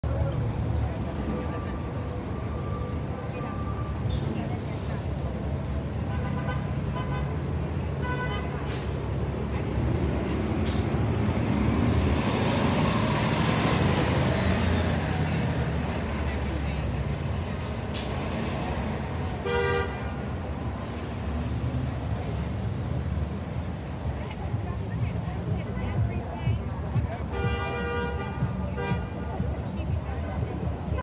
MTA BUS: 2015 New Flyer Xcelsior XD40 [7386] Q20A Bus @ Sutphin Blvd & Archer Avenue/Sneak Peek PT 1
Sneak Peek in the video Part 1